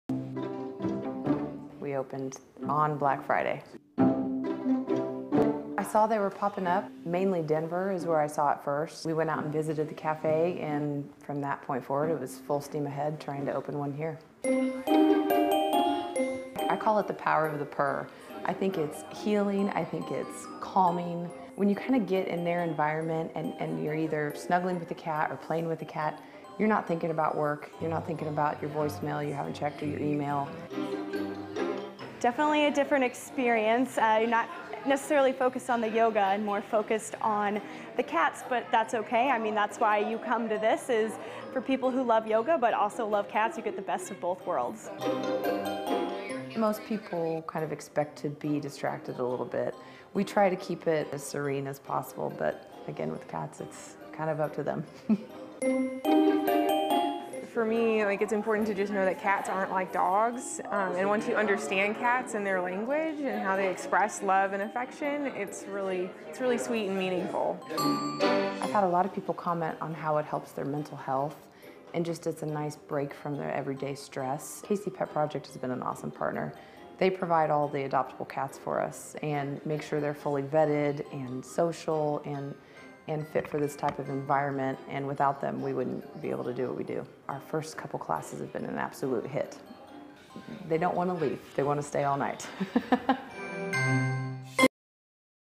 Yoga with cats
Kansas City`s new cat cafe is taking relaxation to a new level -- cat yoga!